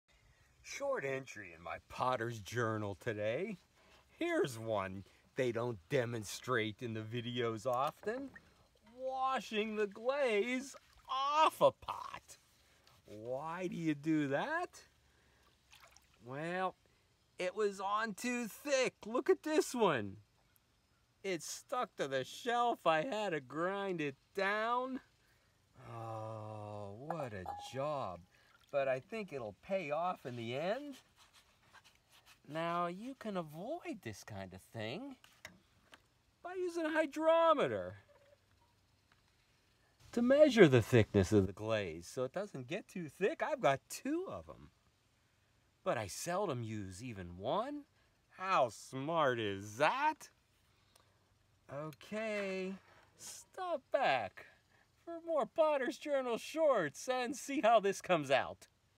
Pottery glaze removal ?????
Ceramic glaze to thick??? Wash it off and start again.